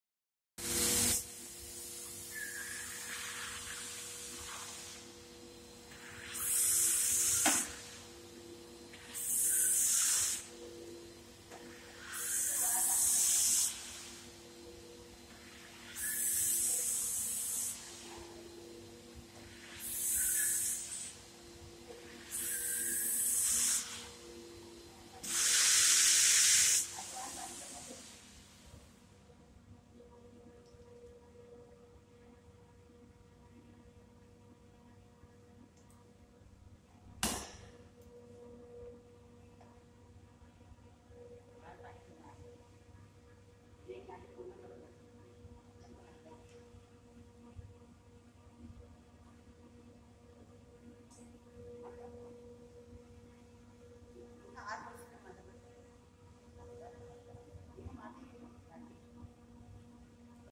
Multipurpose Floor Cleaning Machine - Cyclon Super 17
Single Disc Scrubbing Machine - Super 17
• Larger brush size for faster cleaning.
• Equipped with triple planetary gear transmission system for high torque.
• Centrally mounted motor for balanced operation and control.
• Excellent for all floor & carpet maintenance operations like scrubbing, marble polishing,
   crystallisation, grinding, floor polishing, carpet shampooing,  buffing, scarifying, sanding etc.

Soma Specialities Pvt Ltd.
Address           : 108, Vardhaman Complex, L. B. S. Marg, Vikhroli West, Mumbai,
                            Maharashtra - 400 083, India.